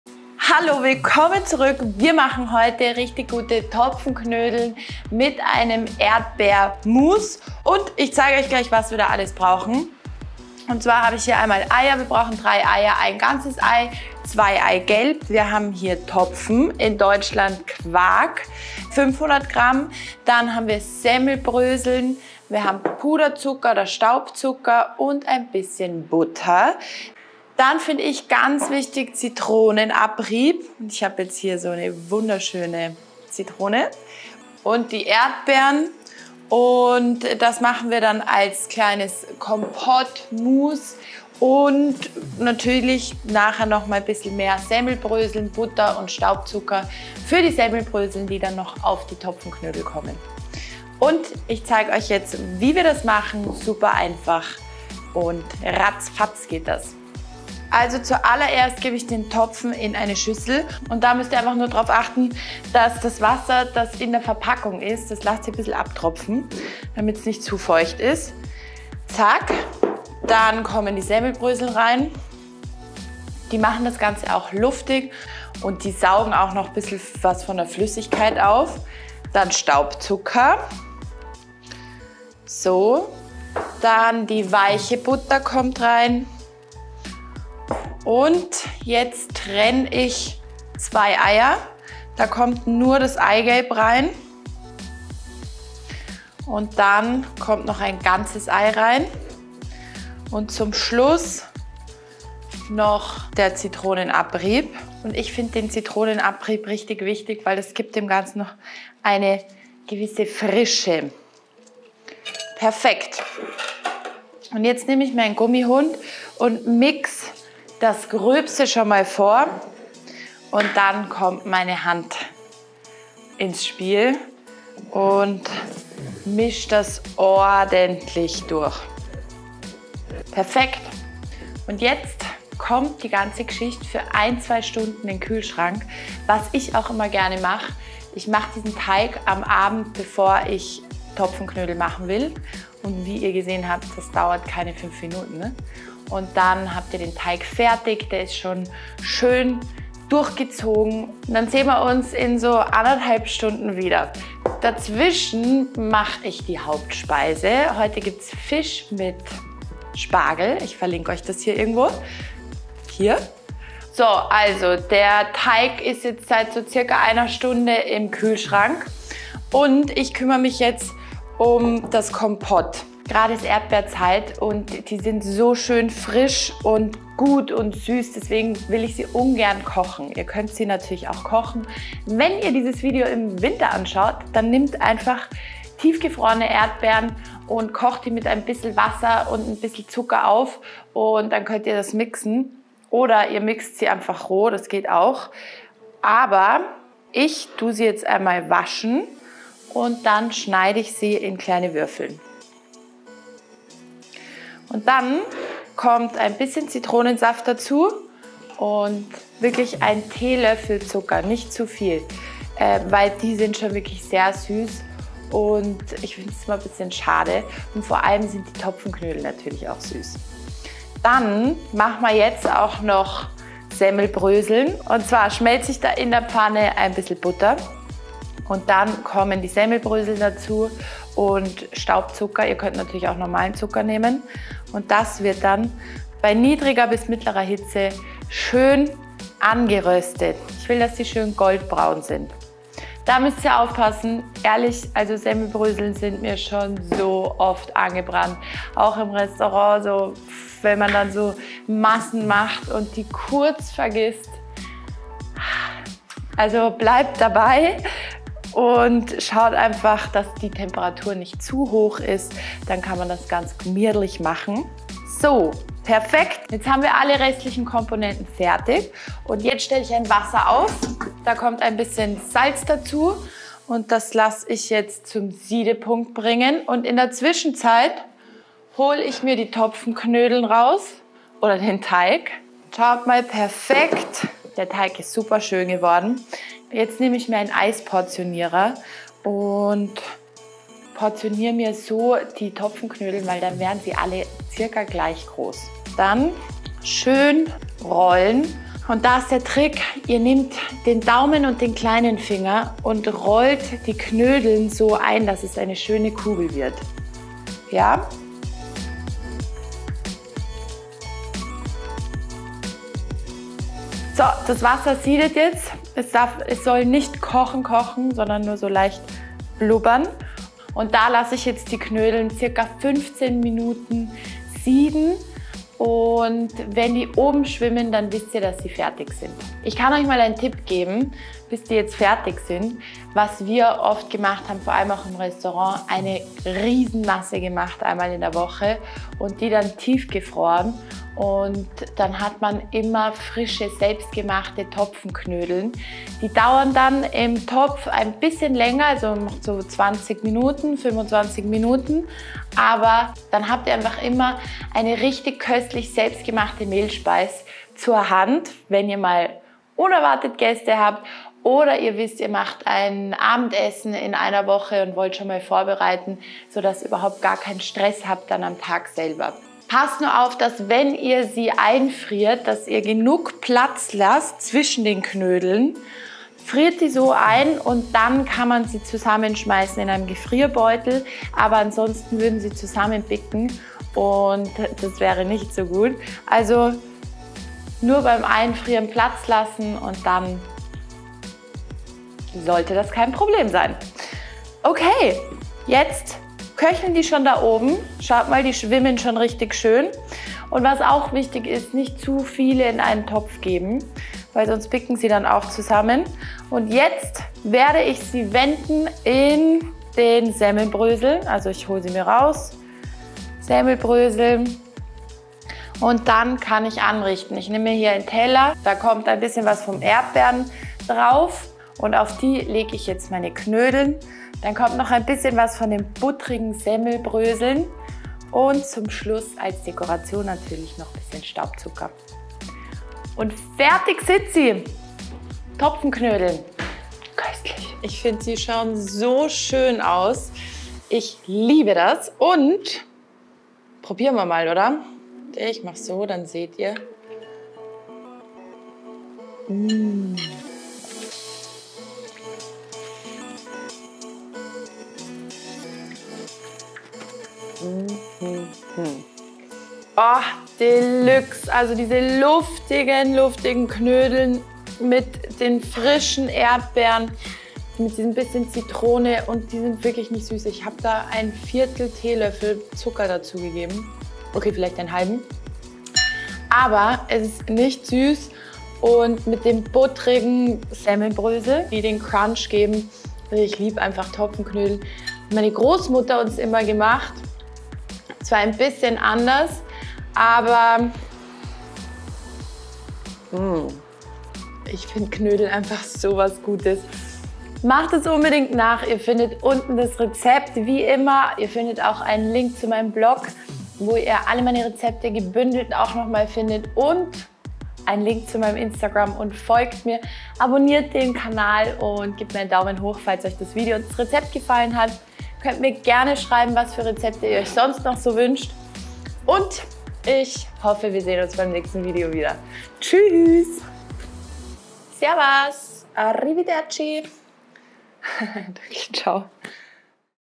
So gelingen sie jedes Mal: ultra weiche Topfenknödel mit Erdbeeren
Topfenknödel gehen bekanntlich immer. In der Sommerzeit esse ich sie aber liebsten auf frischen Erdbeeren. Ideal zum Vorbereiten und Einfrieren für faulere Tage. Das Rezept ergibt ca. 12 Knödel

findest Du jede Woche neue Rezepte, Hacks und Insights aus meiner Küche!

DAS BRAUCHST DU:
250 g frische Erdbeeren
1 TL Zucker
Minze (optional)
Saft einer halben Zitrone
500 g Topfen
90 g Semmelbrösel
60 g Staubzucker
1 Ei
2 Eigelb
40 g weiche Butter
Zitronenabrieb
Butter
Semmelbrösel
Zucker

SO WIRD'S GEMACHT:

1 Den Topfen, Semmelbrösel, Staubzucker, die Eier, weiche Butter und den Zitronenabrieb in einer Schüssel kurz mit einem Holzlöffel vermengen und dann mit der Hand zu einem homogenen Teig kneten.
2 Für mindestens eine halbe bis 2 Stunden in den Kühlschrank stellen und ruhen lassen.

1 In der Zwischenzeit die Erdbeeren in kleine Würfel schneiden und mit Zitronensaft und Zucker mixen. (evntl. noch klein geschnittene Minze hinzufügen.)
2 Butter in einer Pfanne schmelzen, Semmelbrösel und Butter hinzufügen und auf mittlerer Hitze goldbraun rösten.
3 Jetzt die Topfenknödel formen. Als Hilfe ist ein Eisportionierer ideal. Damit werden die Knödel alle gleich groß.
4 Einen Topf mit Salzwasser zum Sieden bringen und die Knödel für ca. 20 Minuten sieden lassen, bis sie obenauf schwimmen.
5 Jetzt in den gerösteten Semmelbrösel wälzen und mit den Erdbeeren anrichten.